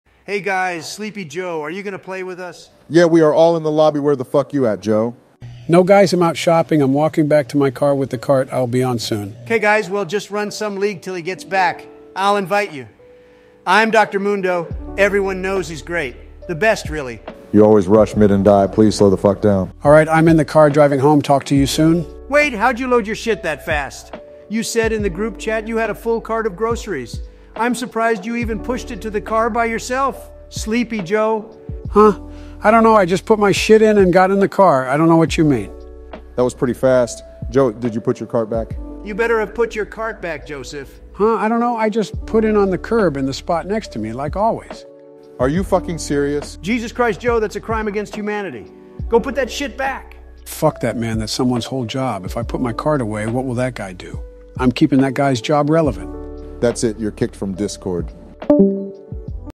BIDEN GETS CAUGHT BY CARTNARCS
DONALD TRUMP AND BARRACK OBAMA ROAST BIDEN WHO DIDNT PUT HIS SHOPPING CART AWAY

SATIRE
VOICE AI